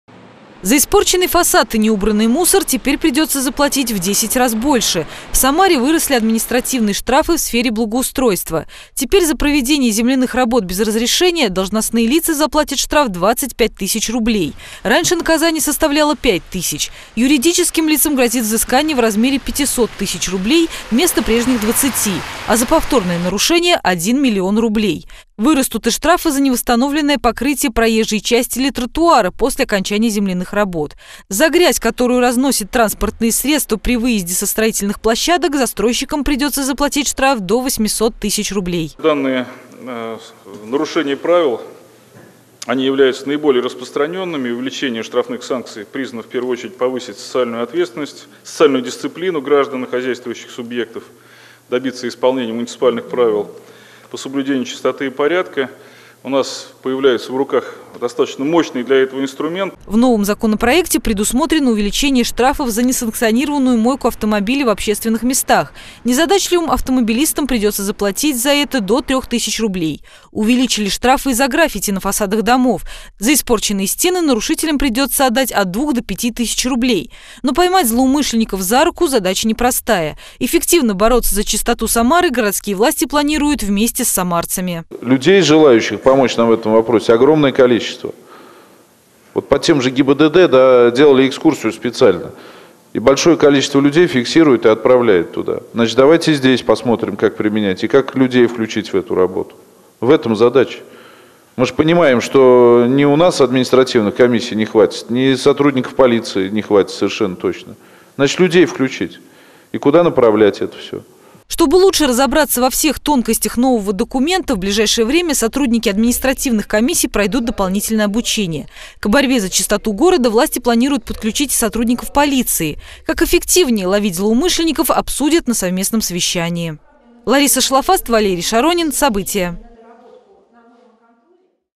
Рублем накажут за нарушения в сфере ЖКХ
В Самаре выросли административные штрафы. Губернская Дума одобрила законопроект о несоблюдении правил благоустройства. Теперь штрафы увеличатся в среднем в 10 раз, а в некоторых случаях и в 50. Как городские власти планируют бороться за чистоту города — обсудили на рабочем совещании в мэрии.